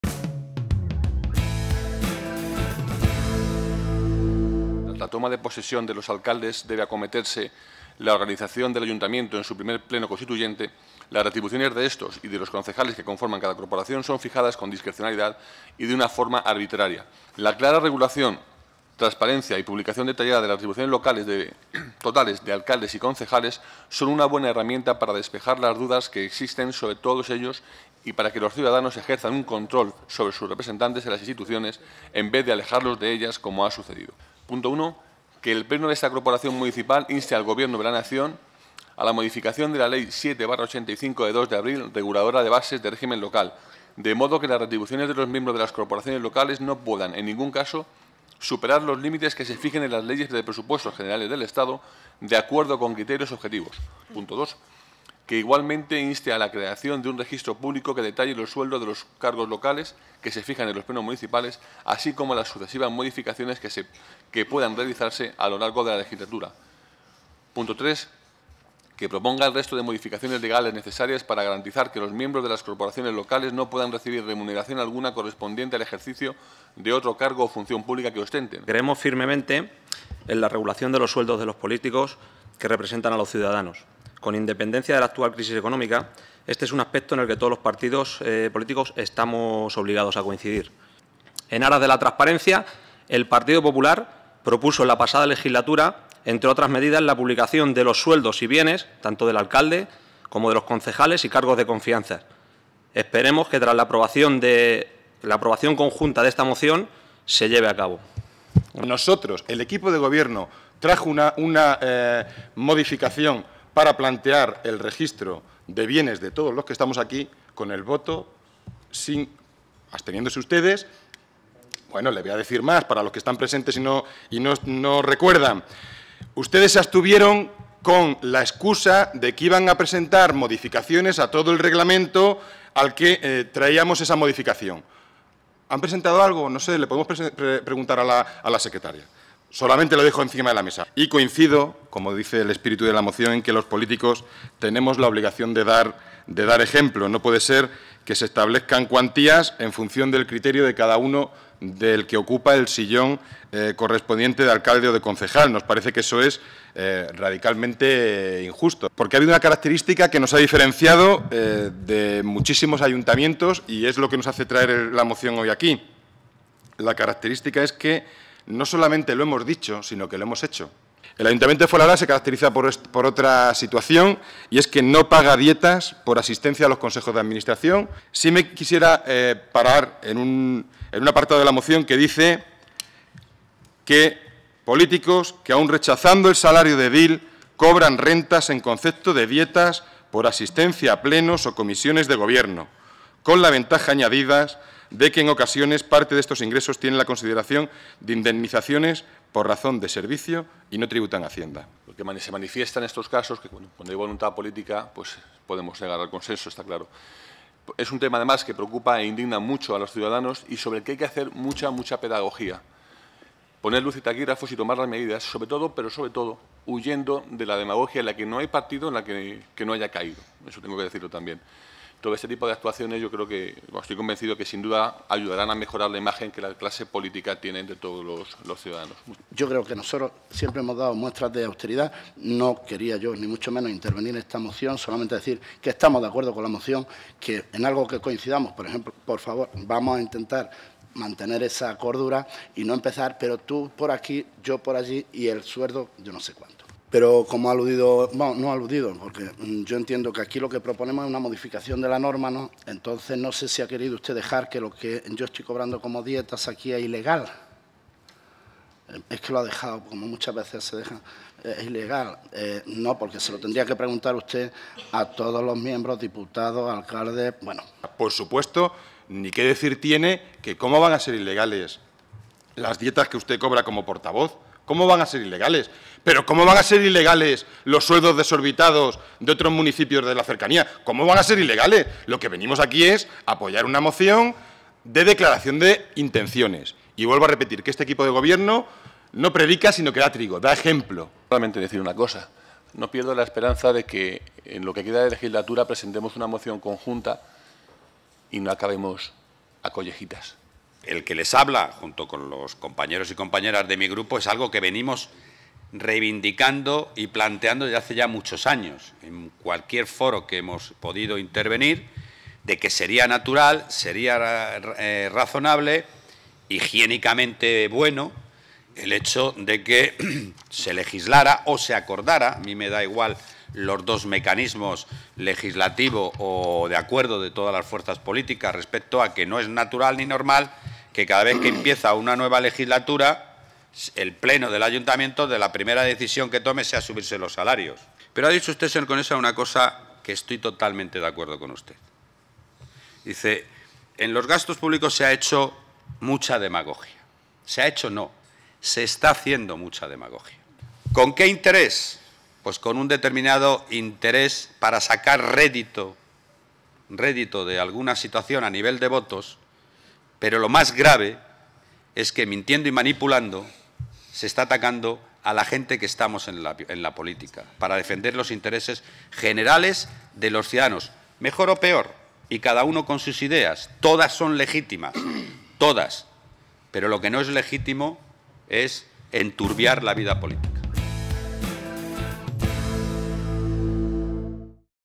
PLENO 5 DE JULIO 2012: moción para limitar los salarios de Alcalde y Concejales
Todos los grupos presentes en el Pleno Municipal aprobaron por unanimidad esta moción en la que se recogían una serie de reivindicaciones referentes a las remuneraciones de los miembros de las corporaciones municipales, entre ellas la homogeneización de salarios, la incompatibilidad de remuneraciones, el registro público de los sueldos, etc.

Se trata de una reivindicación que el Gobierno Municipal lleva poniendo en práctica desde hace años, siendo Fuenlabrada uno de los municipios de España de su nivel con los salarios más bajos, incluyendo el del propio Alcalde, y donde además no se cobran dietas por asistencia a consejos de administración de empresas públicas y organismos autónomos.